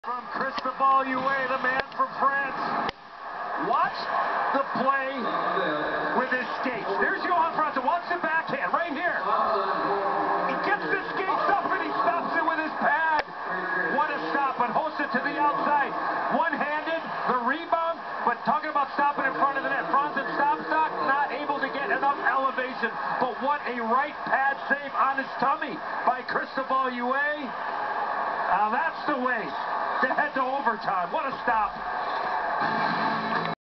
Red Wings vs. Blackhawks NHL Playoffs 2009 Game 5 Incredible Save
Christobal Huet of the Chicago Blackhawks making an incredible save against Johan Franzen during game 5 of the 2009 playoffs to send it in to overtime.  I am a Detroit fan, but this save was insane.